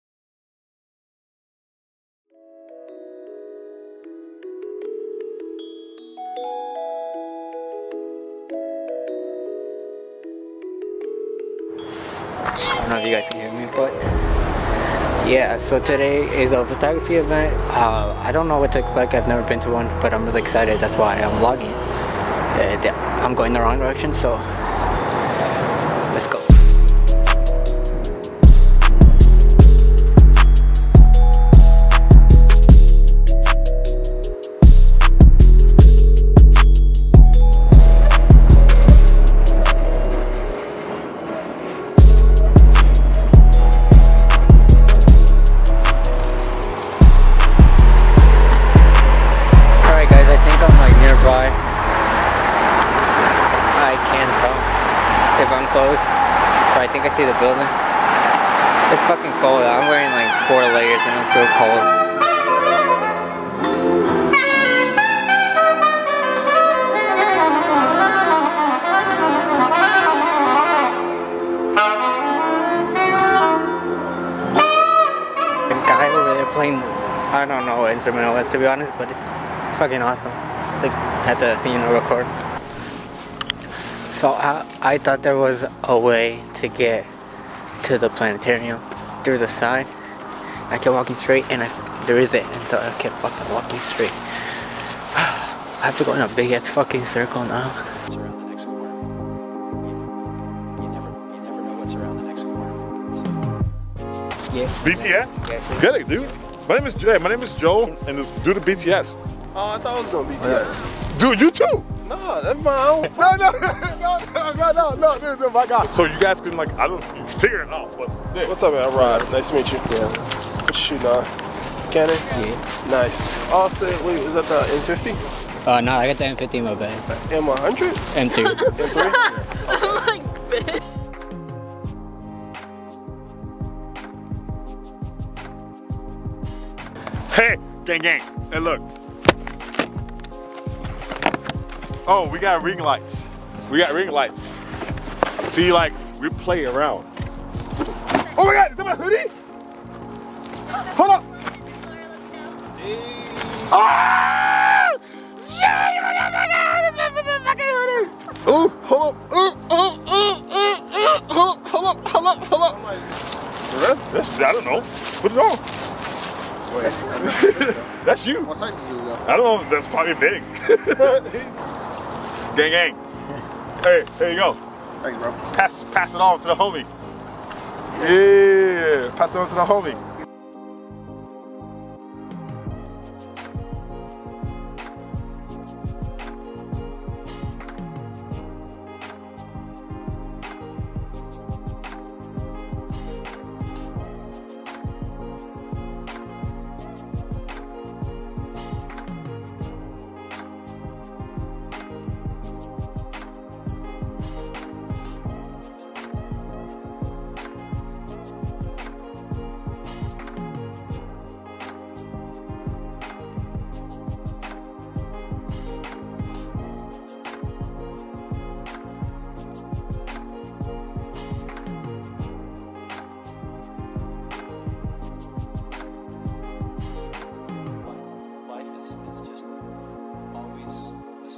Clever Aperture Photography MEET Up Vlog #8!!
Hey guys today I am vlogging the the time I went to a meet up. this was suppose to be my first video back into youtube as you can tell not well filmed. stay inside guys stay safe

Everyone else's Instagram
Film makers
Me
Wolfmedia

Photographers
Clever Aperture
Chriz D. M.

Models
Hayatte